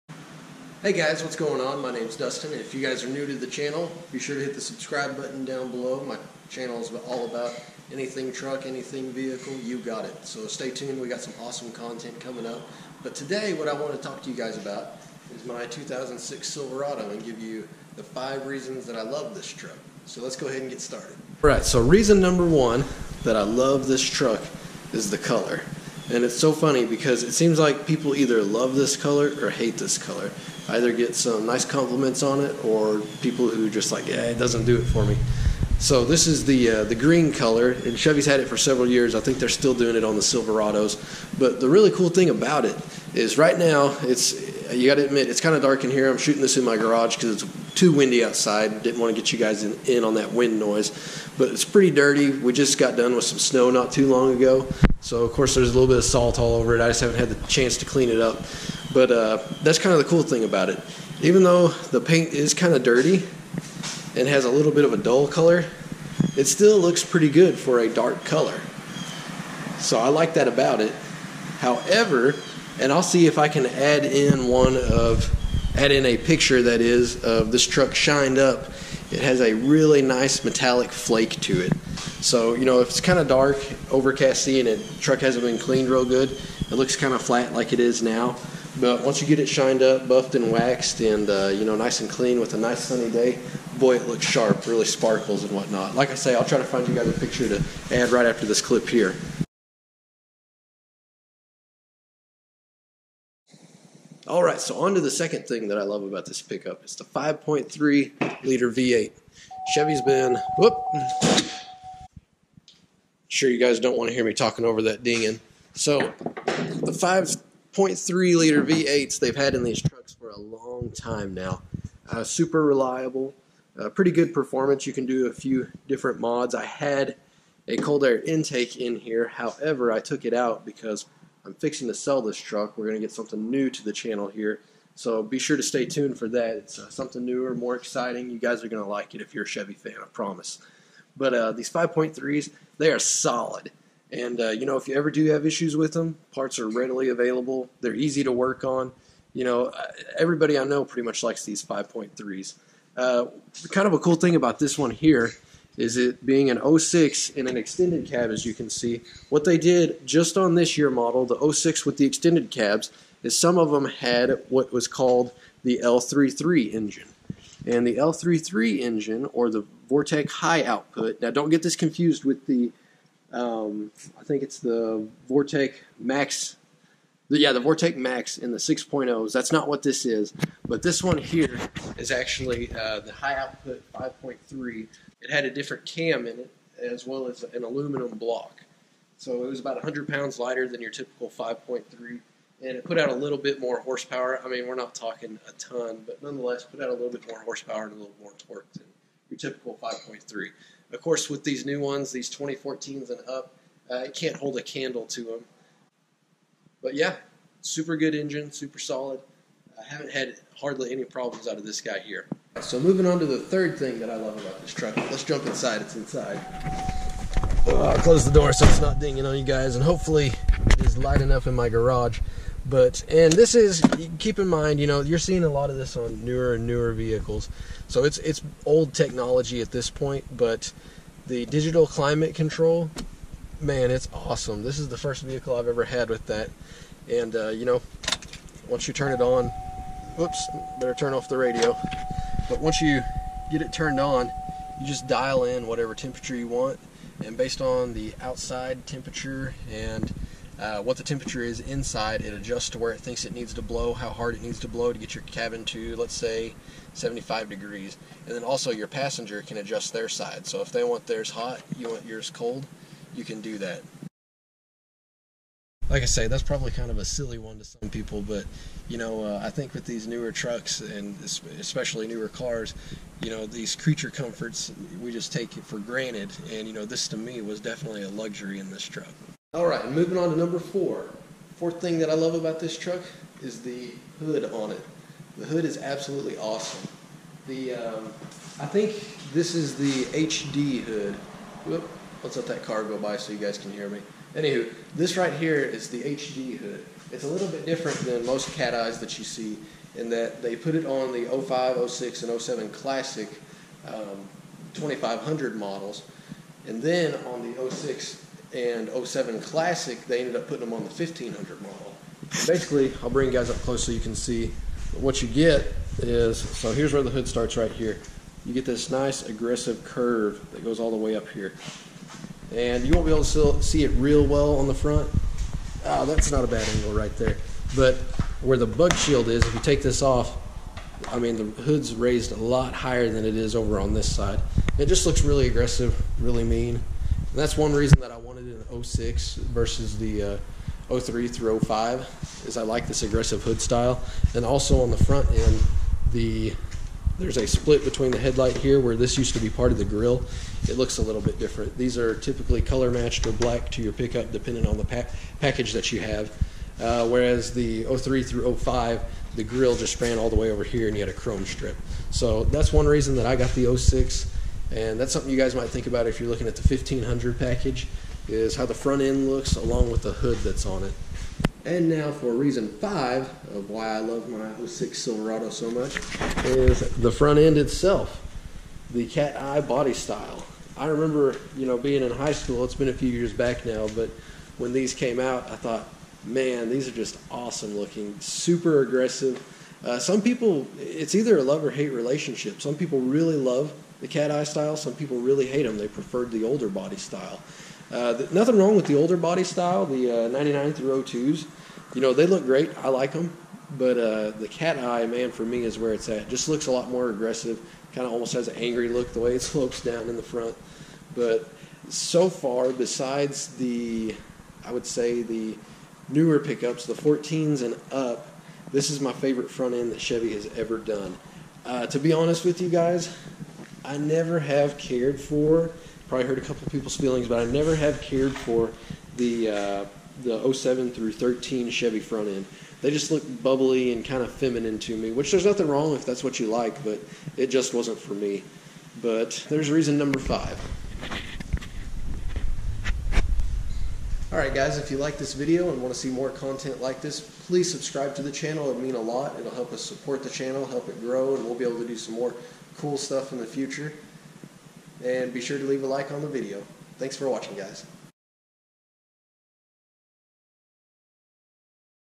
2006 Silverado (5 Things I LOVE)
5 reasons why I like my Chevy Silverado 2006